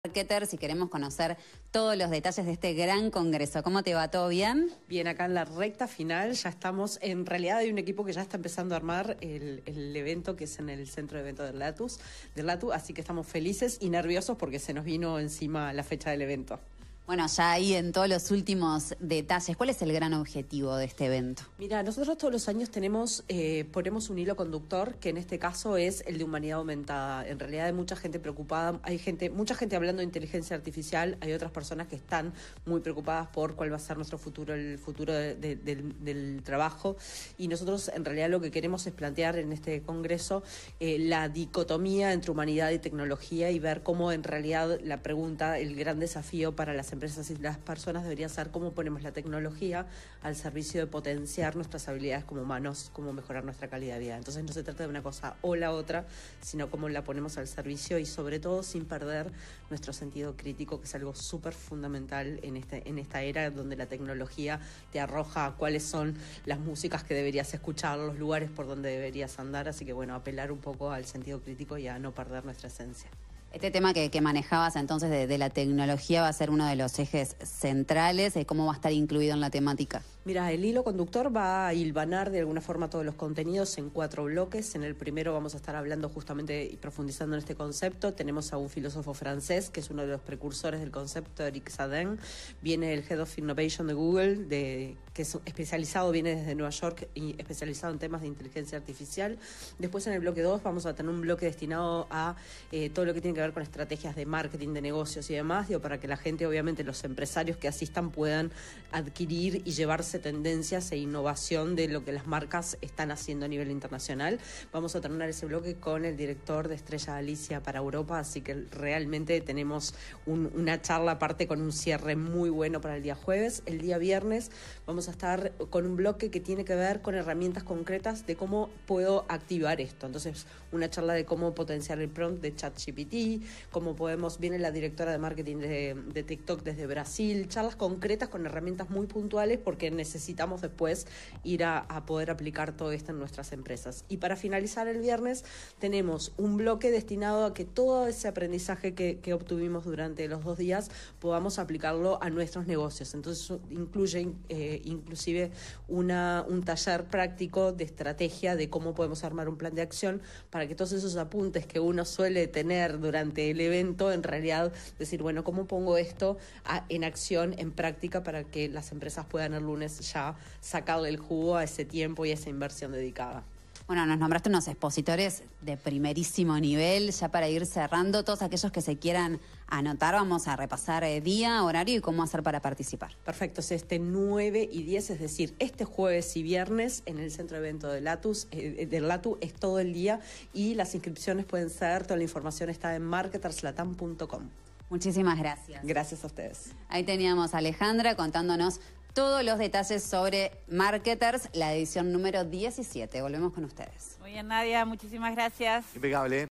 Marketers, congreso de management, marketing y comunicaciones
Nadia Fumeiro conversó con Alejandra Pradere, CEO de Marketers Latam.